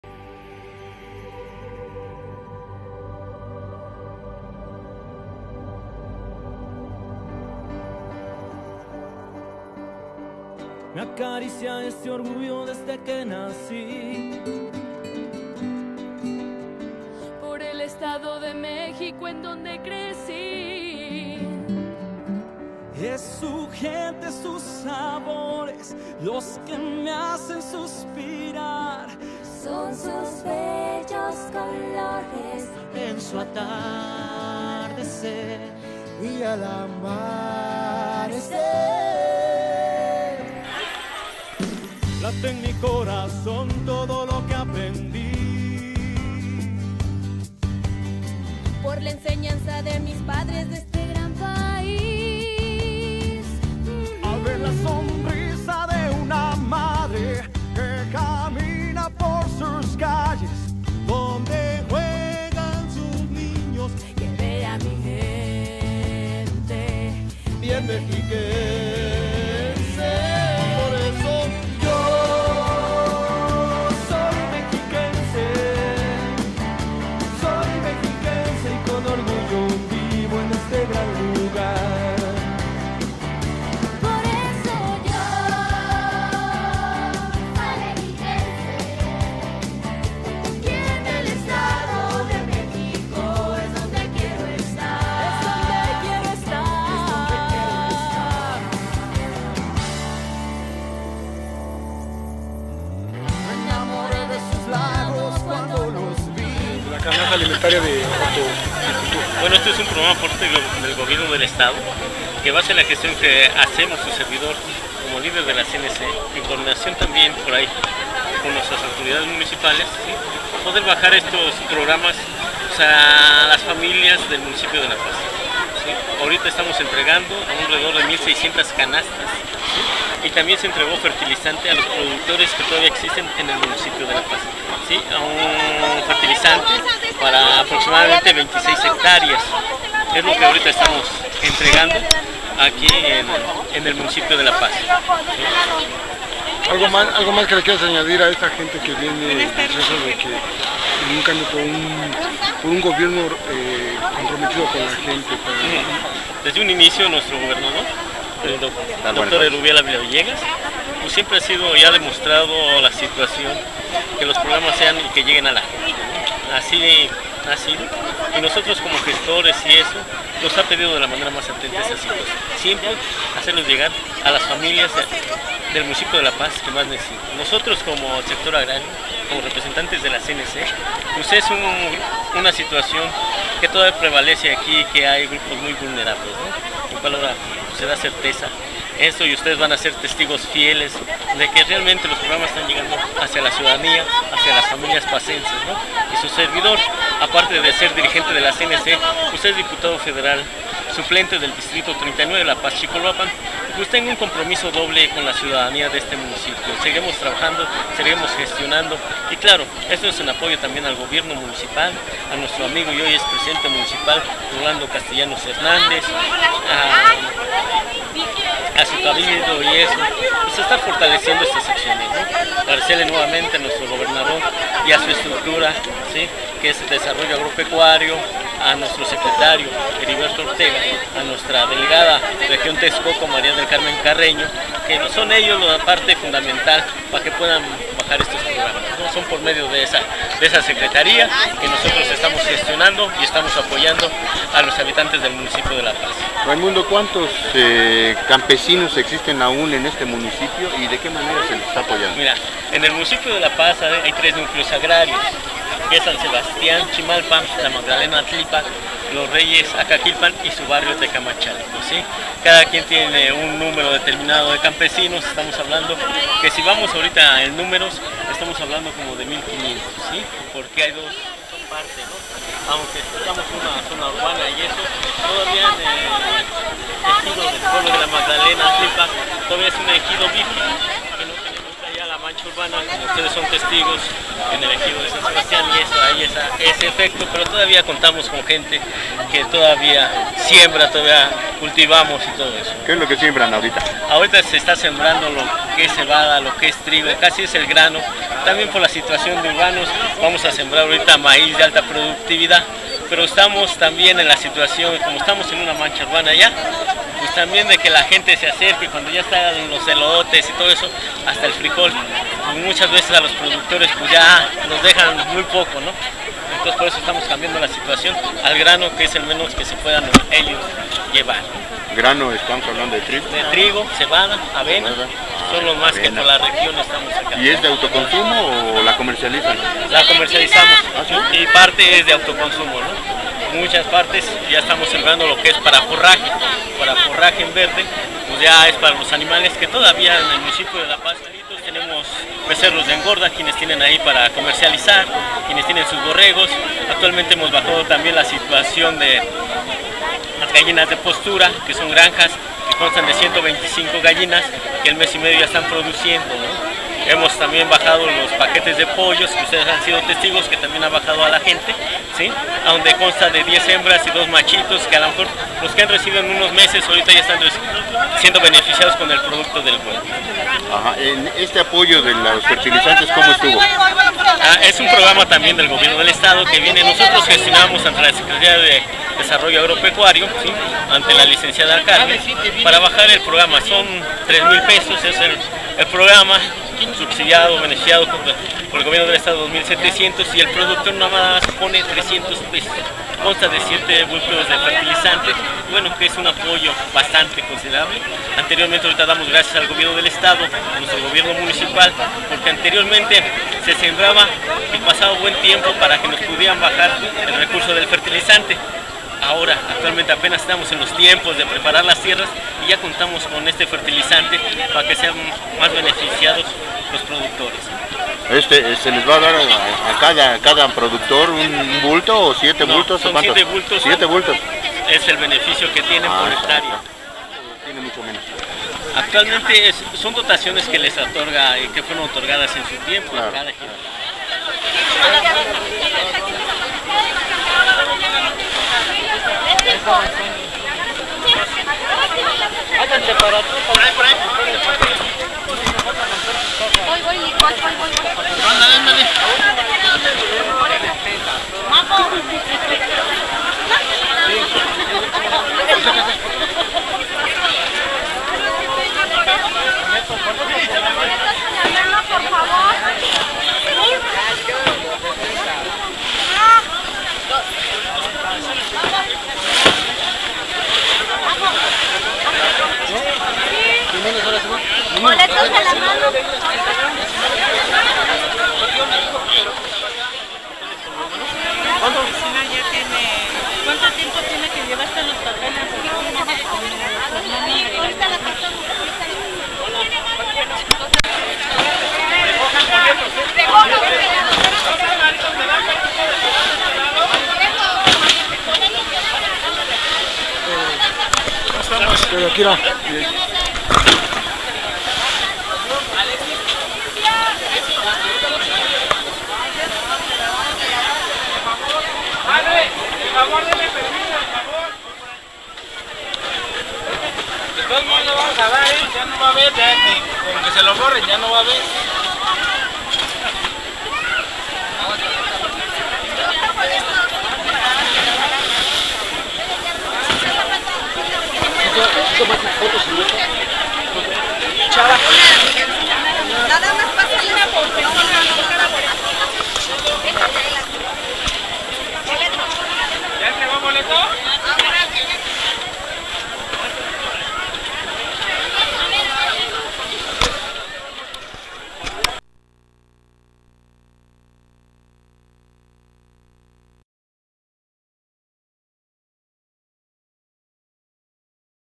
CNC MAGDALENA ENTREGA 1600 CANASTAS
Acompañado de nuestro Presidente Municipal Rolando Castellanos Hernández, por la Delegada de la Sedagro Edomex en la región Texcoco Lic. María del Carmen Carreño García, la representante del gobierno mexiquense Carla Libertad Domínguez del Rio y el líder cenecista y Diputado Federal Suplente Raymundo Moreno Ibáñez efectuó la entrega de canastas hortofrutícolas y fertilizante químico del Programa de Insumos Agrícolas 2017 gestionado ante el Gobierno del Estado de México